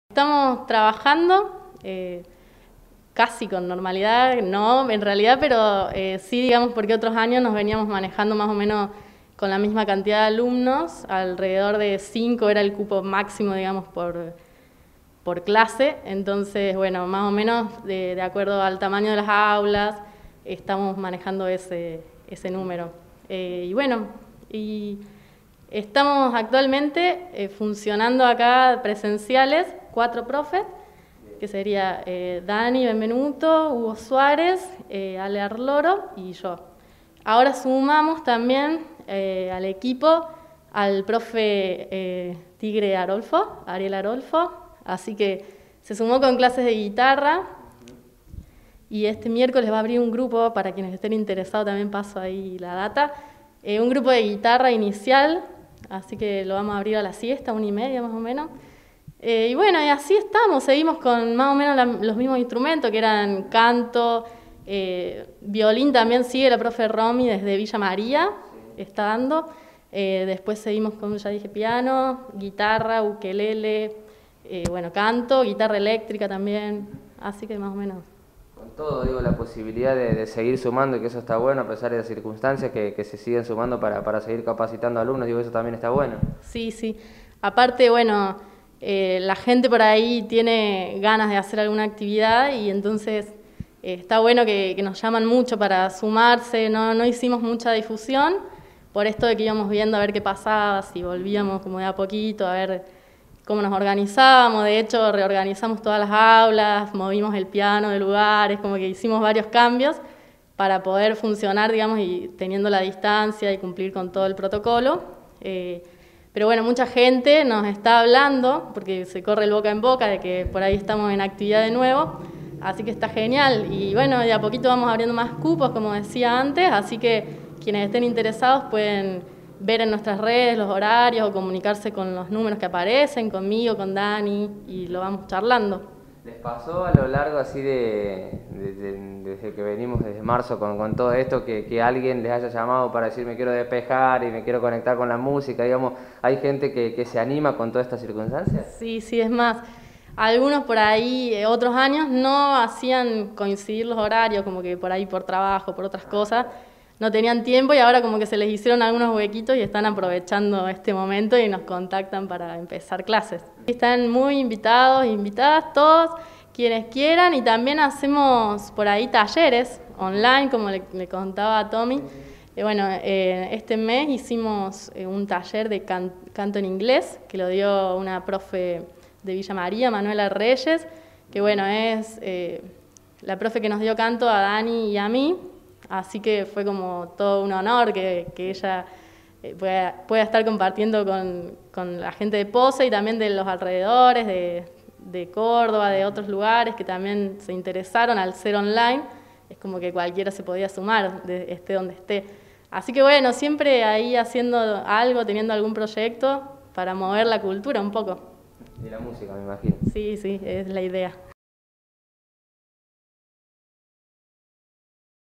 Lisette Lujan - In Crescendo
MÚSICA PARA SANAR

Recorrida por la comunidad, buscando las expresiones culturales vinculadas al aprendizaje en tiempos de pandemia. Hoy, visitamos In Crescendo, escuela de formación musical con clases presenciales, virtuales y talleres para vivir el mundo de la música.